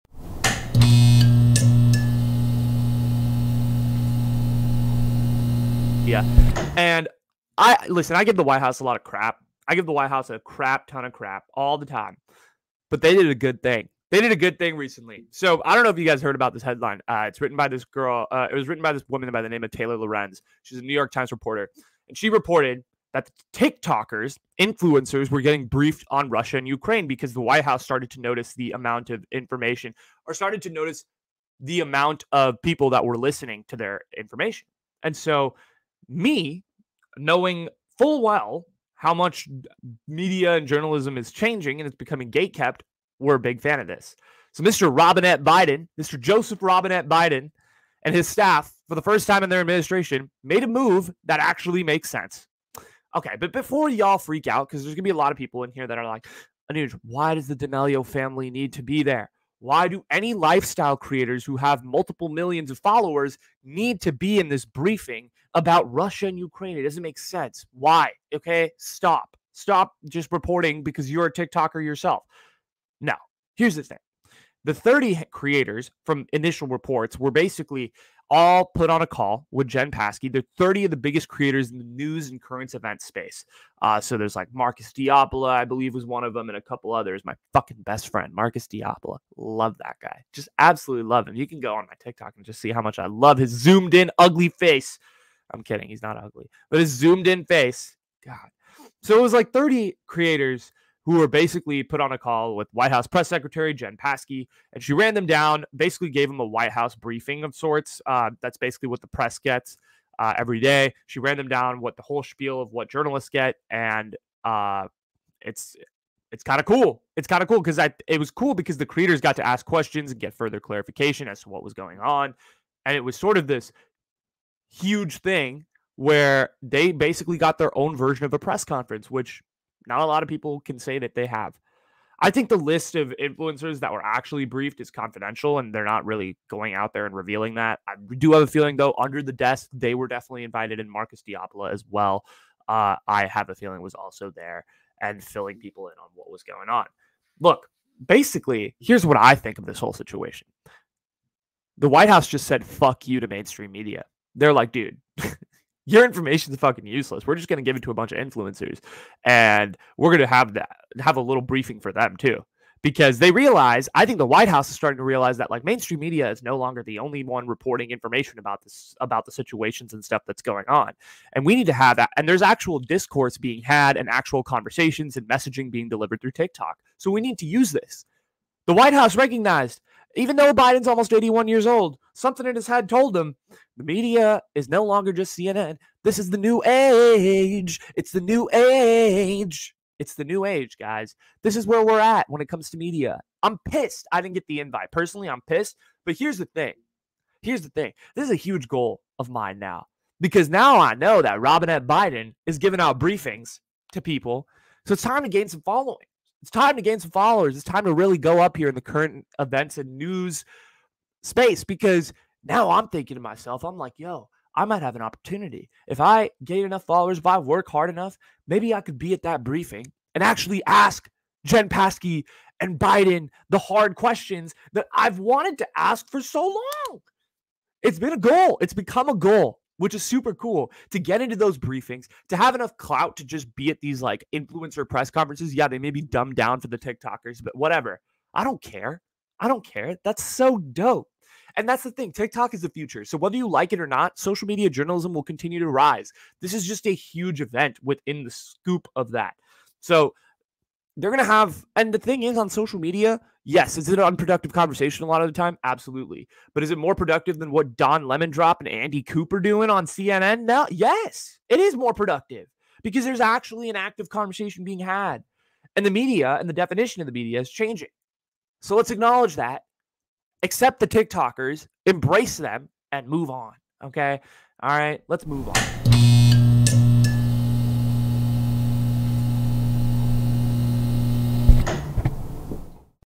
TikTokers Are Now Journalists?!
Anuj talks about the new development that Tiktokers were briefed on Russia and Ukraine, and how this could be the future of journalism.